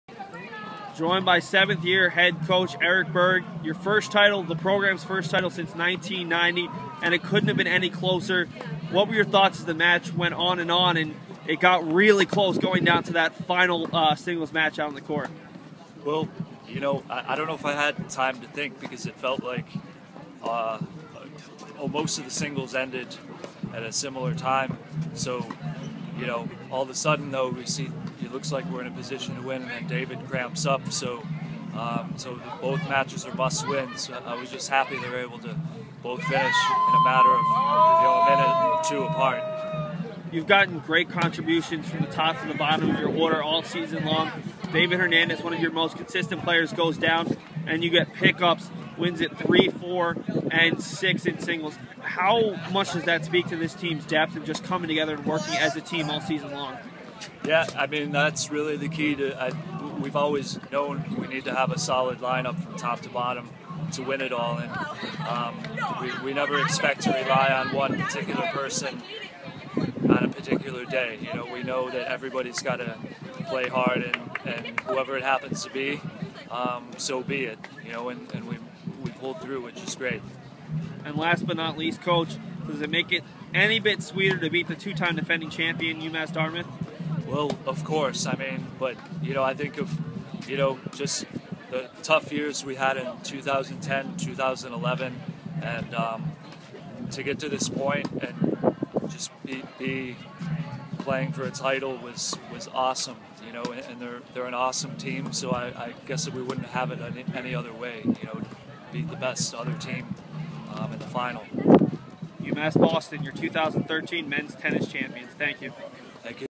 UMB Tennis Coach Eric Berg Little East Conference Championship Postgame Interview (5/4/13)
UMass Boston head coach Eric Berg talks about his team's Little East Conference Championship performance as the Beacons outlasted defending champion UMass Dartmouth 5-4. The championship was the first in Berg's seven-year tenure and first for UMB in tennis since 1990.